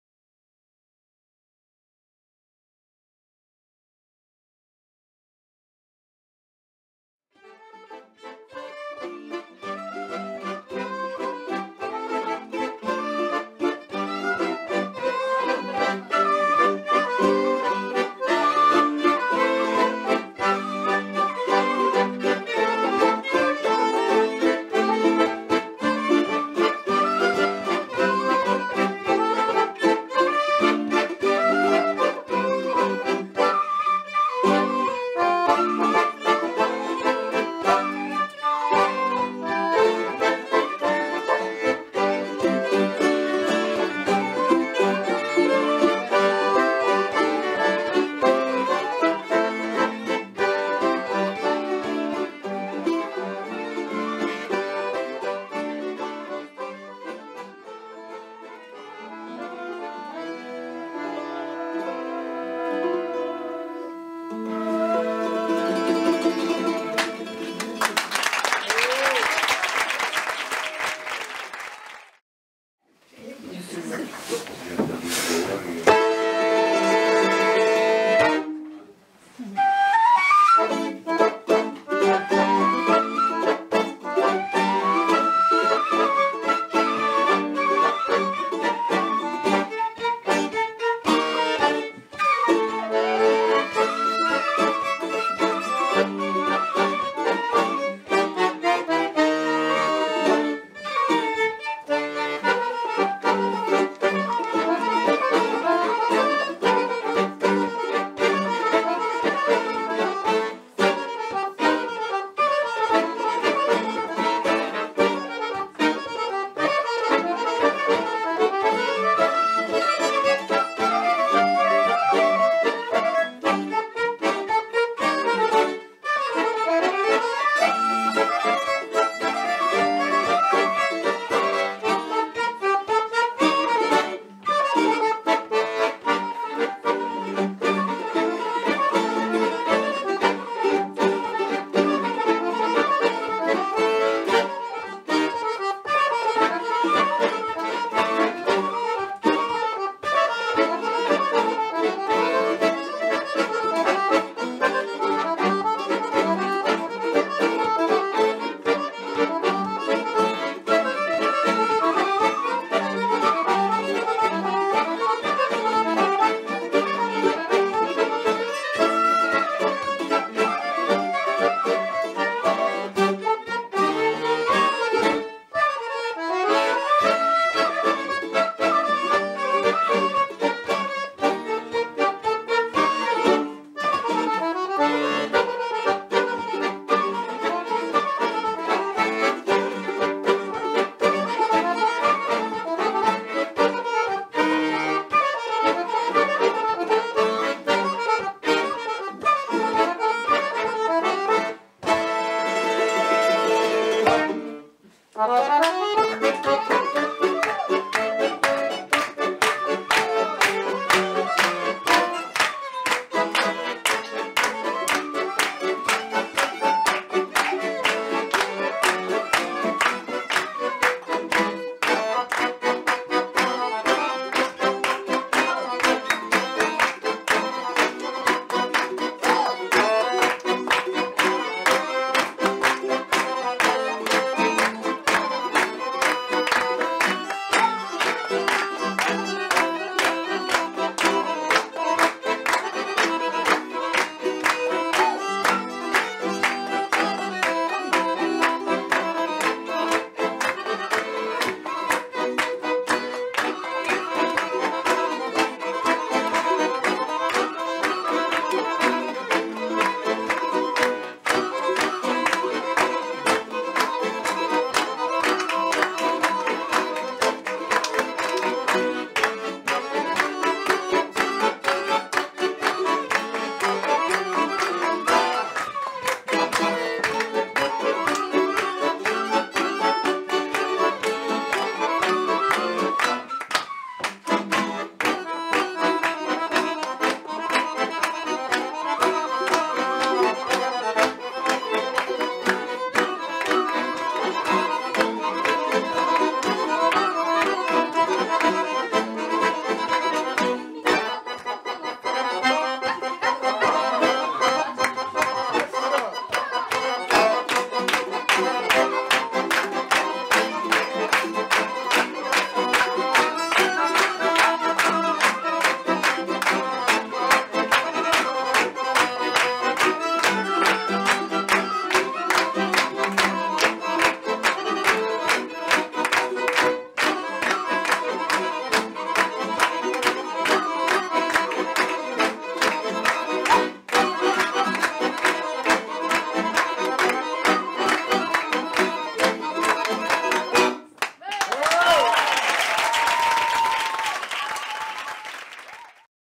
MishMash Bulgarian Folkproject 2016
Concert in France 2016 with MishMash Band. Performing some bulgarian folksongs.

Violin: Hannah Sundkvist (Sweden)
Kaval: Milena Saxena (Norway)
Accordian: Yanush HURWITZ (Israel)
Tambura: Diez Pascal (France)